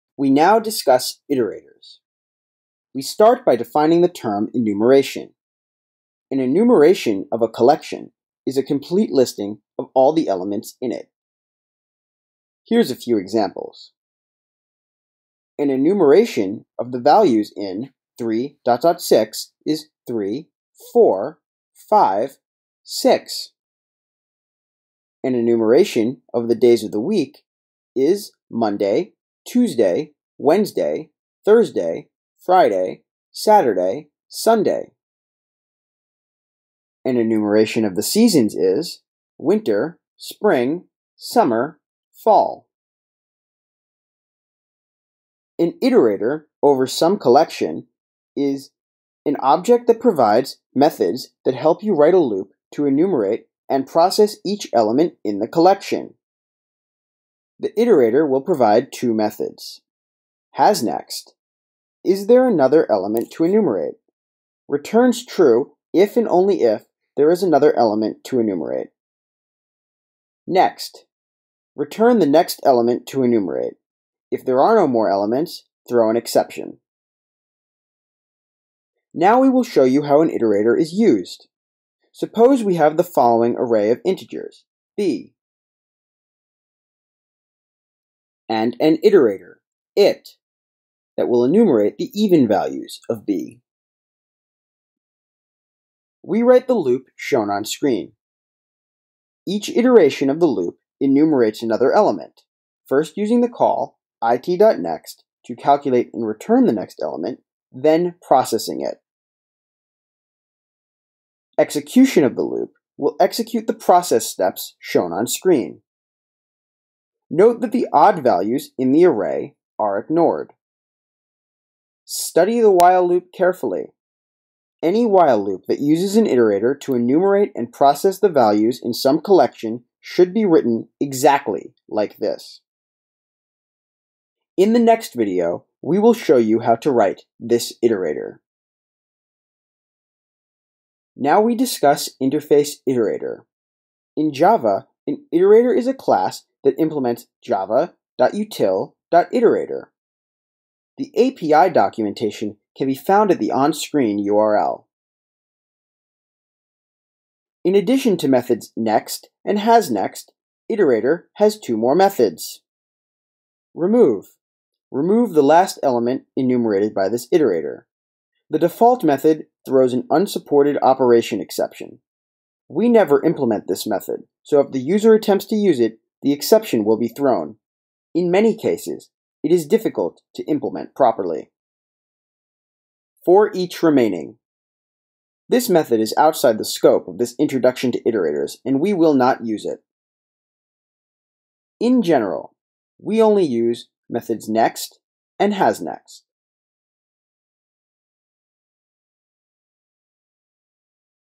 Iterator/Iterable 1. Interface Iterator
We explain the word "enumeration" and then introduce interface Iterator. An object that maintains a collection (set, list, etc.) and implements interface Iterator contains methods to help enumerate the collection.